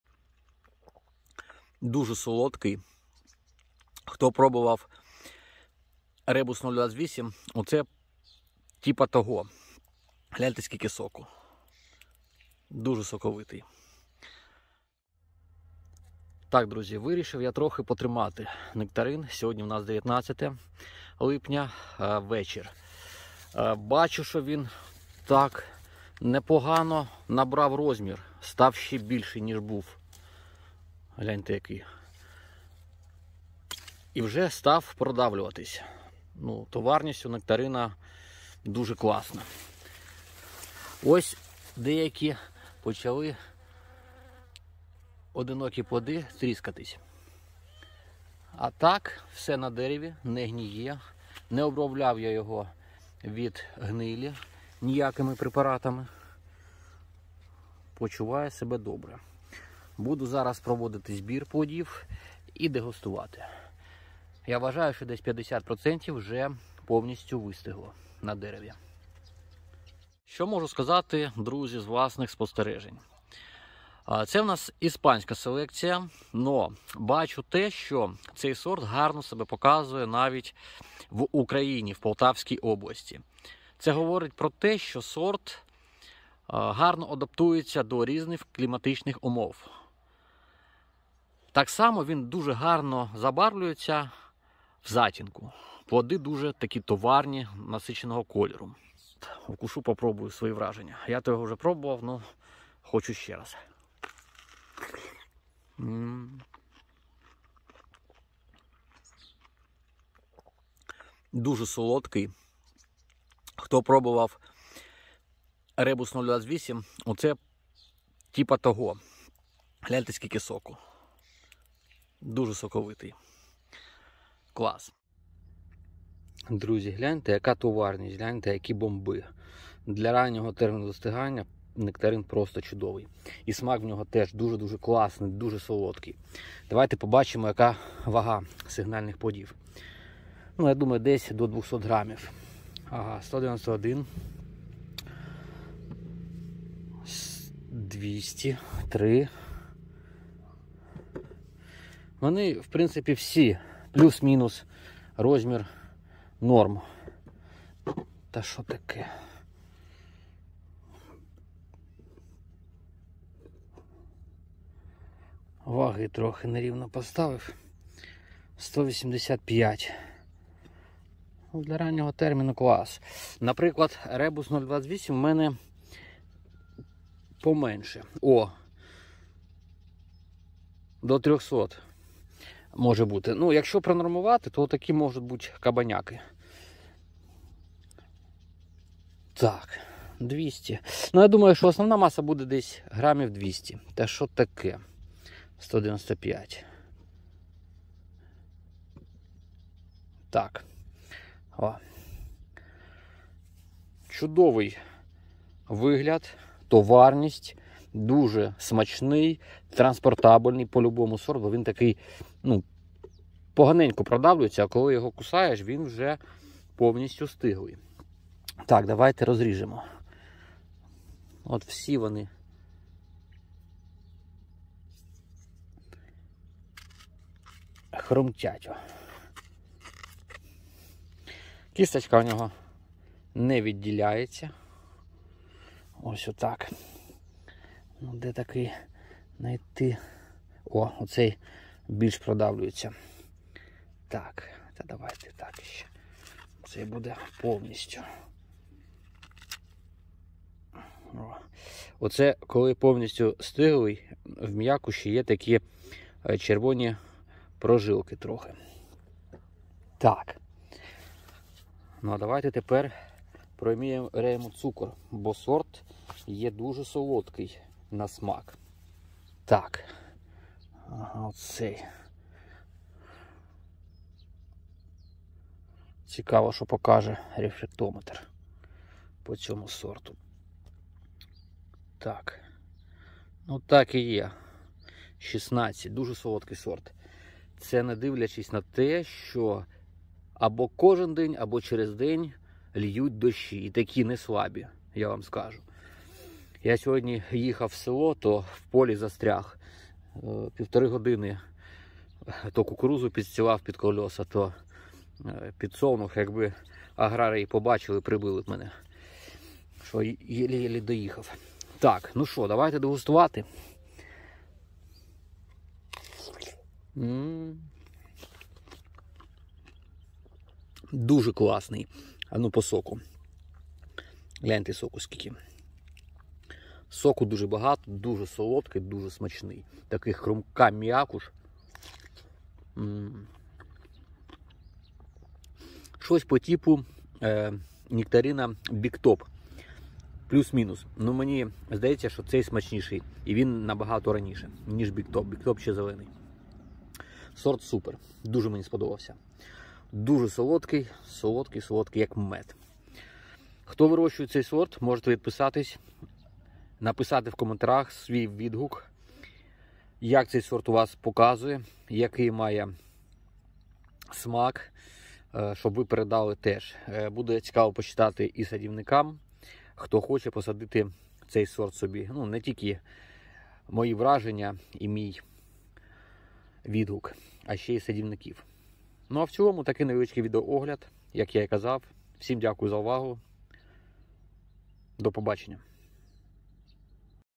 Нектарин Екстрім Бьюті / Nectarine Extreme Beauty
Нектарин Екстрім Бьюті Extreme Beauty- новий дуже ранній та цінний нектарин селекції Іспанії.
Плоди великі, масою 200 г, діаметром близько , темно-червоні, майже бордові, звичайної кулястої форми. Забарвлення не змінюється в залежності від розташування деревини в кроні. Усі плоди одномірні, добре забарвлені. М'якуш дуже щільний, твердий, при повному поспіванні м'якшає, дуже соковитий і солодкий, хрумкий. Плоди відмінно транспортуються та зберігаються. Дозрівання плодів повільне після набирання забарвлення дозволяє розтягувати технічний період для транспортування, до моменту споживання і повної стиглості. Зовні плоди дуже красиві, елегантні, насиченого забарвлення.
Смак дуже добрий, ароматний, переважає дуже солодкий смак. Сорт відрізняється дуже якісними і є одним із ранніх сортів нектаринів ринкового призначення.